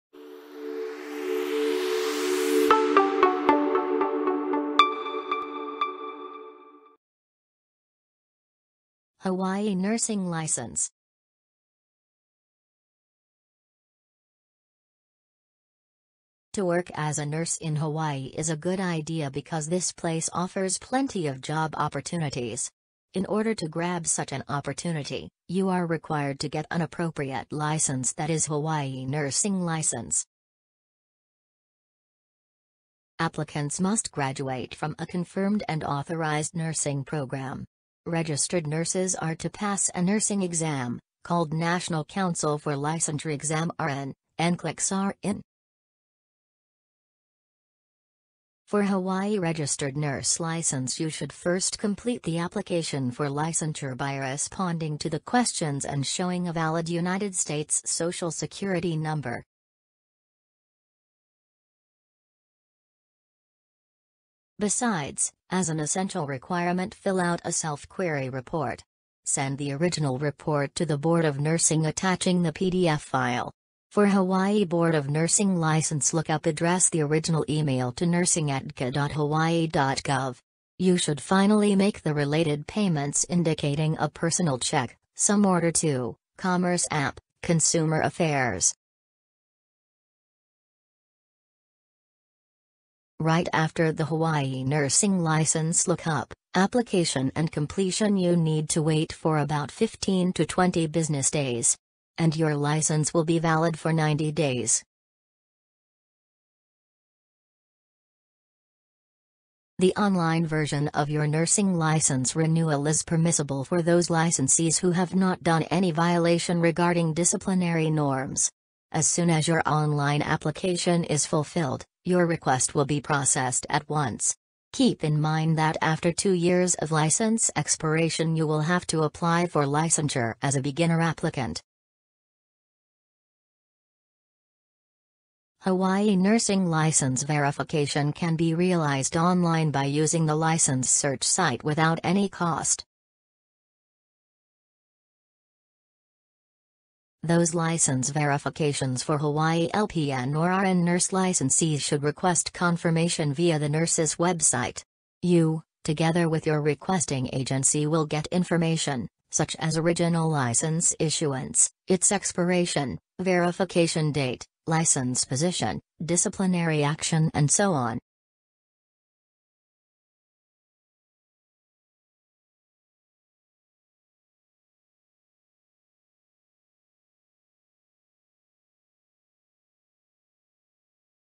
Hawaii Nursing License - What You need to get started
On our page, you’ll get the latest detailed information on getting a Hawaii Nursing License.  Here you can find how to apply for your license